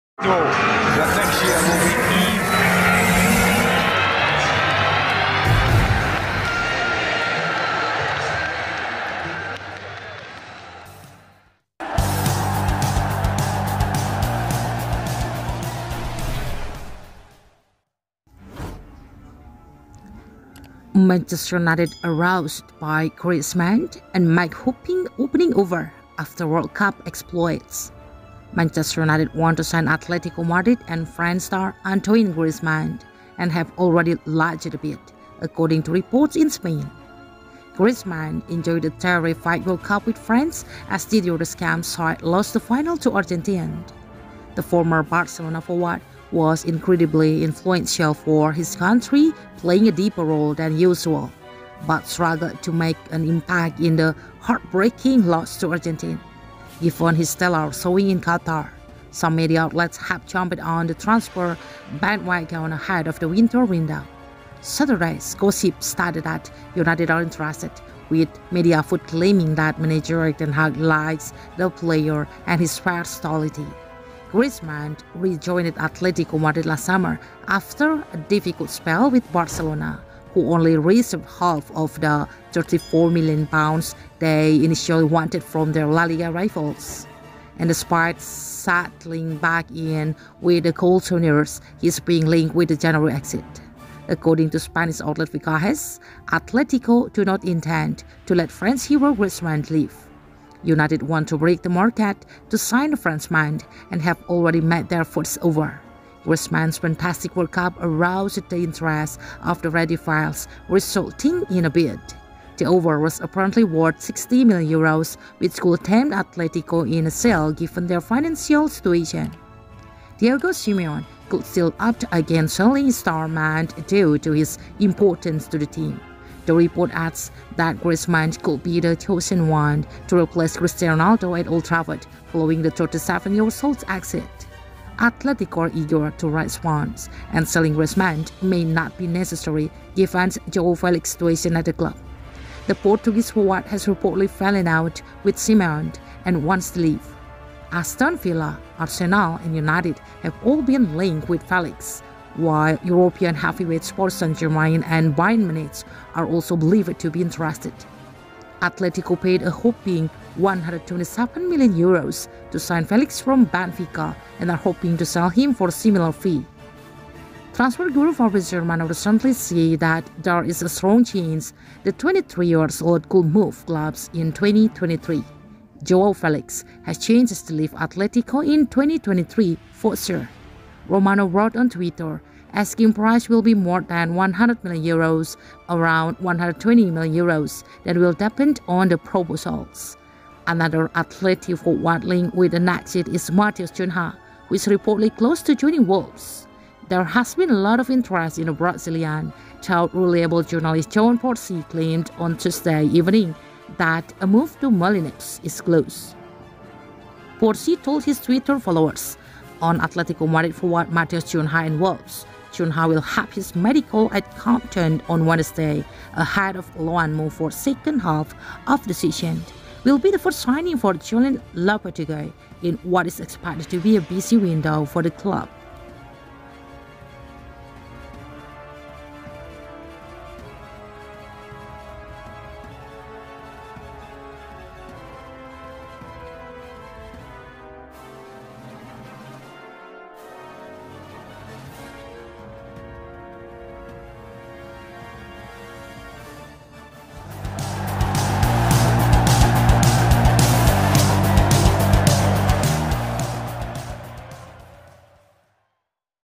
NEW NUMBER 7: Man United ‘aroused’ by forward star and make whopping ‘opening offer’
Man Utd ‘aroused’ by Griezmann and make whopping ‘opening offer’ after World Cup exploits

Manchester United want to sign Atletico Madrid and France star Antoine Griezmann and have already lodged a bid, according to reports in Spain.

Griezmann enjoyed a terrific World Cup with France as Didier Deschamps’ side lost the final to Argentina.

The former Barcelona forward was incredibly influential for his country playing a deeper role than usual, but struggled to make an impact in the heartbreaking loss to Argentina.

Given his stellar showing in Qatar, some media outlets have jumped on the transfer bandwagon ahead of the winter window.

Saturday’s Gossip stated that United are interested, with Mediafoot claiming that manager Erik ten Hag likes the player and his versatility.

Griezmann re-joined Atletico Madrid last summer after a difficult spell with Barcelona, who only received half of the £34million they initially wanted from their La Liga rivals.

And despite settling back in with the Colchoneros, he is being linked with a January exit.

According to Spanish outlet Fichajes, Atletico ‘do not intend’ to let France hero Griezmann leave.

United ‘want to break the market’ to sign the Frenchman, and have already made their ‘first offer’.

Griezmann’s fantastic World Cup ‘aroused the interest’ of the Red Devils, resulting in a bid.

The offer was apparently worth €60m, which could tempt Atletico into a sale given their financial situation.

Diego Simeone could still opt against selling his star man due to his importance to the team.

The report adds that Griezmann could be the ‘chosen one’ to replace Cristiano Ronaldo at Old Trafford following the 37-year-old’s exit.

Atletico are eager to raise funds, and selling Griezmann may not be necessary given Joao Felix’s situation at the club.

The Portuguese forward has reportedly fallen out with Simeone and wants to leave.

Aston Villa, Arsenal and United have all been linked with Felix, while European heavyweights Paris Saint-Germain and Bayern Munich are also believed to be interested.

Atletico paid a whopping €127m to sign Felix from Benfica and are hoping to sell him for a similar fee.

Transfer guru Fabrizio Romano recently said that there is a strong chance the 23-year-old could move clubs in 2023.

“Joao Felix has chances to leave Atletico in 2023, for sure,” Romano wrote on Twitter. “Asking price will be more than €100m, around €120m – then will depend on the proposals.”

Another Atleti forward linked with an exit is Matheus Cunha, who is reportedly close to joining Wolves.

There has been a lot of interest in the Brazilian, though reliable journalist John Percy claimed on Tuesday evening that a move to Molineux is close.

Percy told his Twitter followers: “On Atletico Madrid forward Matheus Cunha and Wolves: Cunha will have his medical at Compton on Wednesday ahead of a loan move for second half of the season.

“Will be the first signing for Julen Lopetegui in what is expected to be a busy window for the club.”